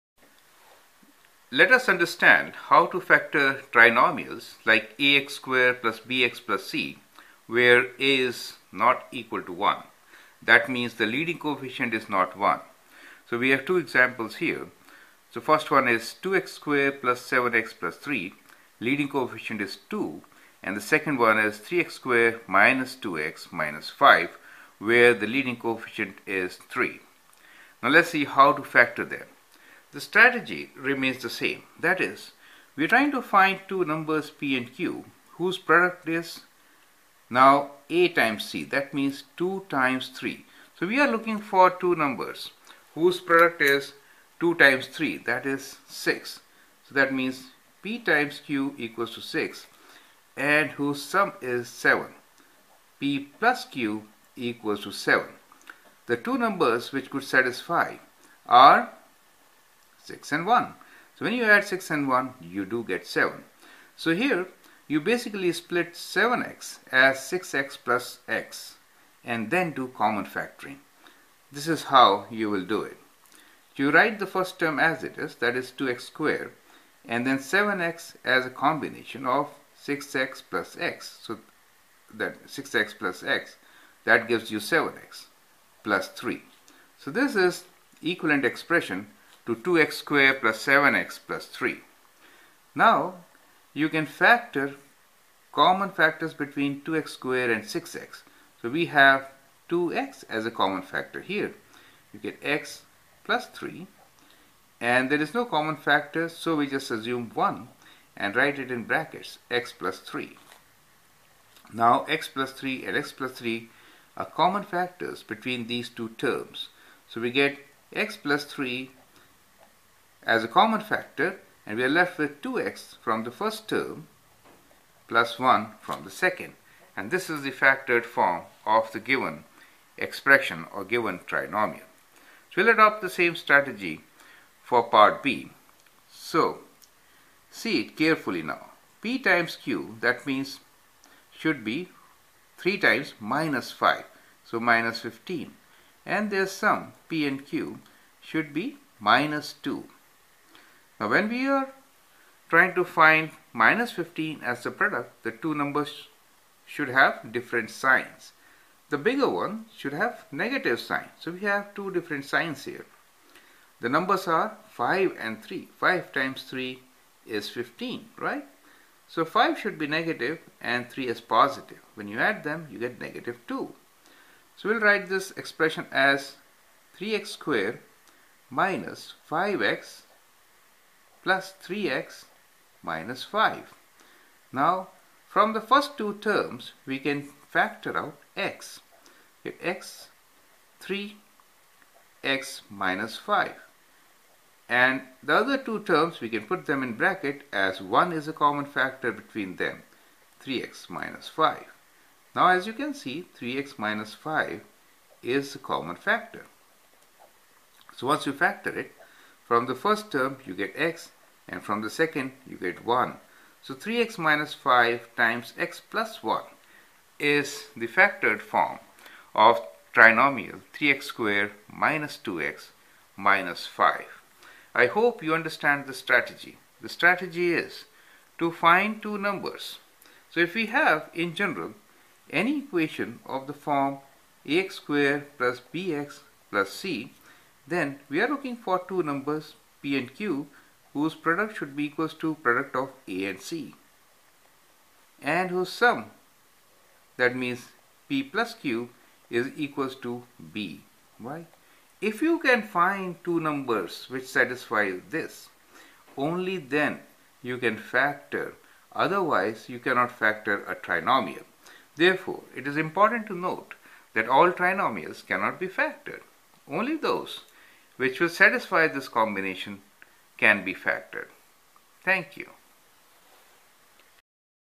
24 Algebra -- Factor Trinomials
Factor Trinomials. to Factor Trinomials of Type ax^2 + bx +  c find two numbers whose product is ac and sum is b. Split the coefficient of x in these two numbers. Then group factor followed by final factoring. Examples help to practice this concept.